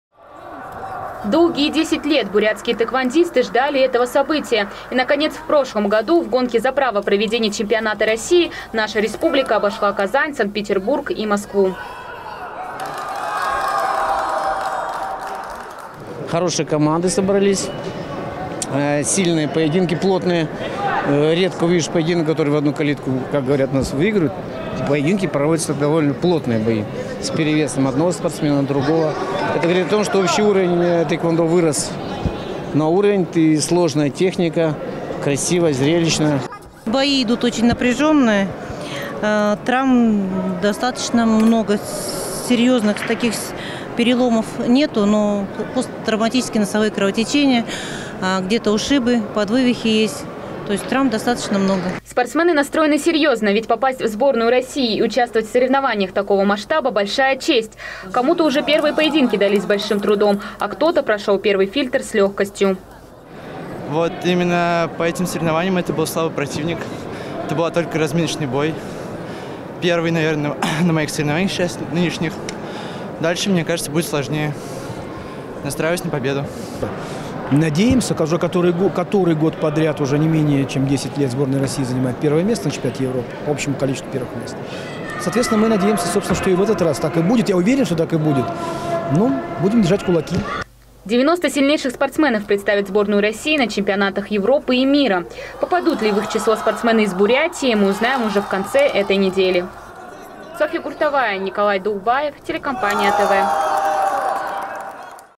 В Улан-Удэ продолжается чемпионат России по тхэквондо ITF
Сотни тхэкквондистов  из 30 регионов схлестнулись в яростных поединках в стенах физкультурно спортивного комплекса. Второй день идет борьба за путевки на предстоящие чемпионаты Европы и мира.   Подробности в репортаже Софьи Гуртовой.